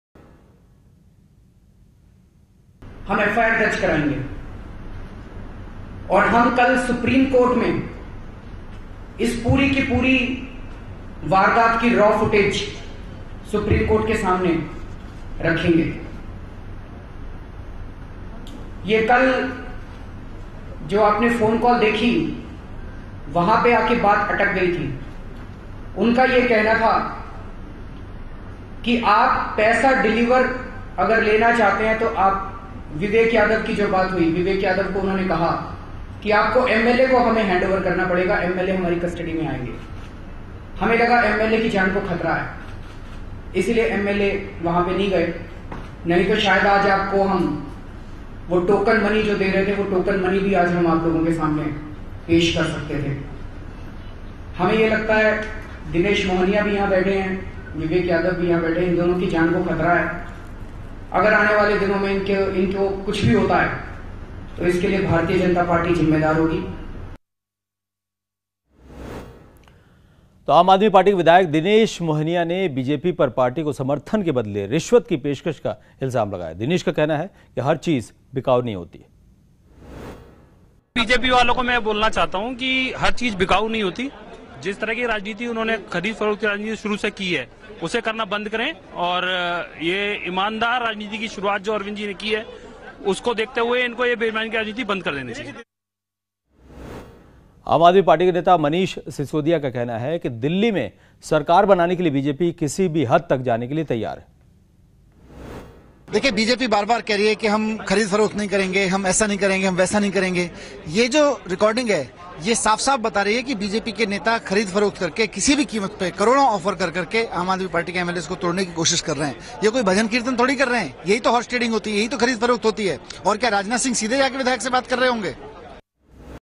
Arvind Kejriwal performs sting to expose BJP
AAP leader Arvind Kejriwal hosted a press conference today and alleged BJP of poaching his MLAs.

He has released a video as a proof to the allegation.